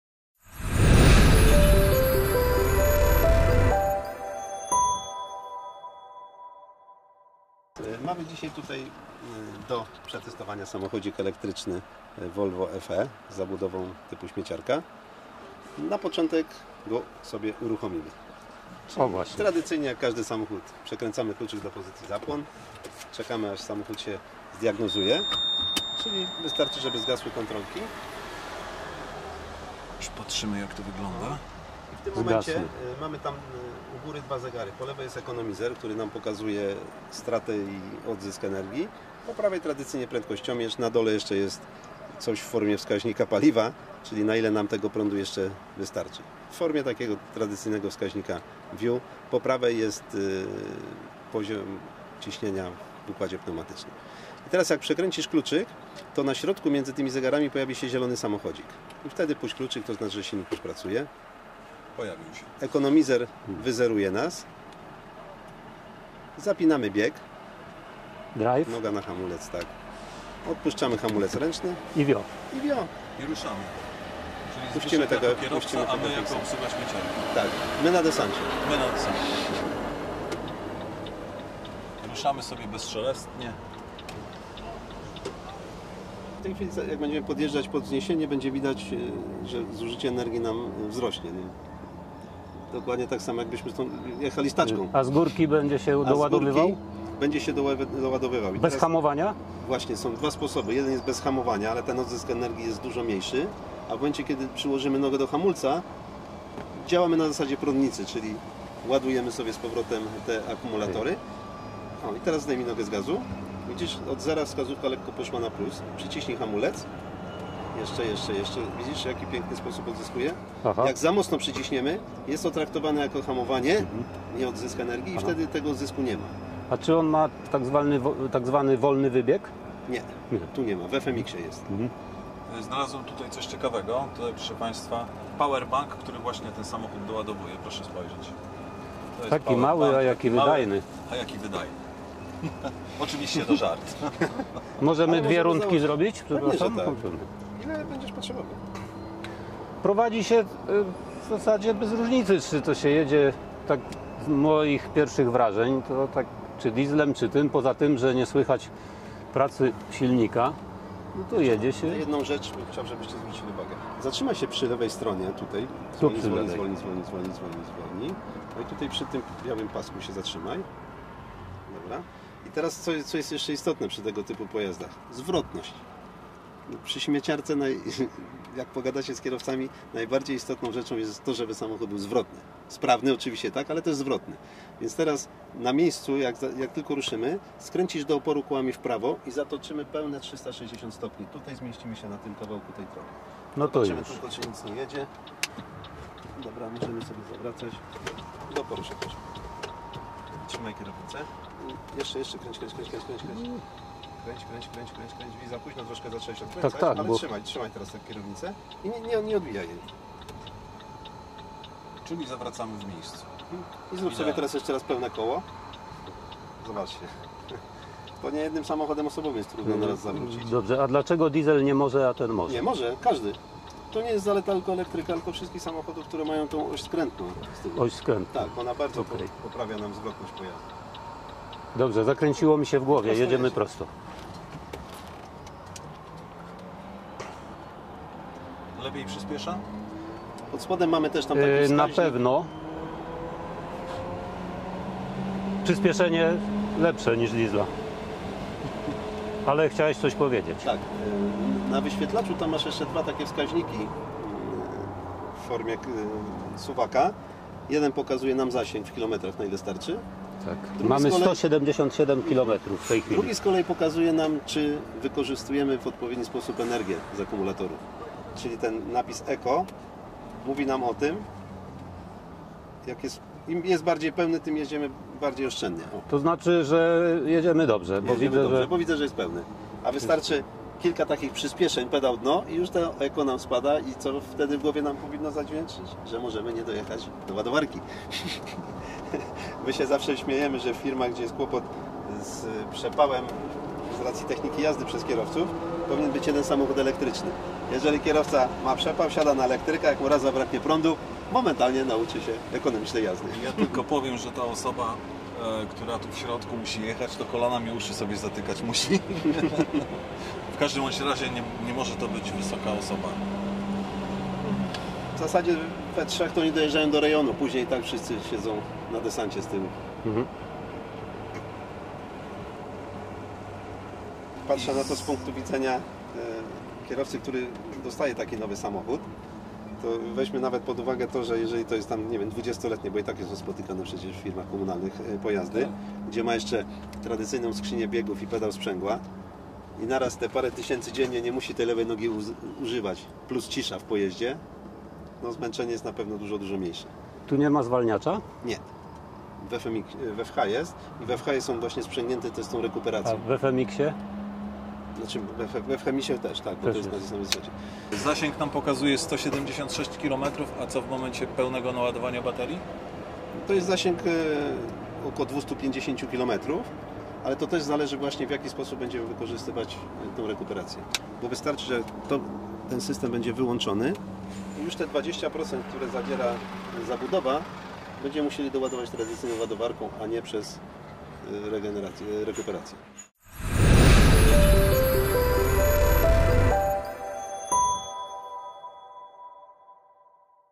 Volvo FE Electric - Ma dobry zasięg!
@volvotrucks @Truck_pl @autodromjastrzab9854 e-truck
Jeździmy elektrycznym Volvo FE. Jak myślicie? Jaki zasięg może mieć taka ciężarówka?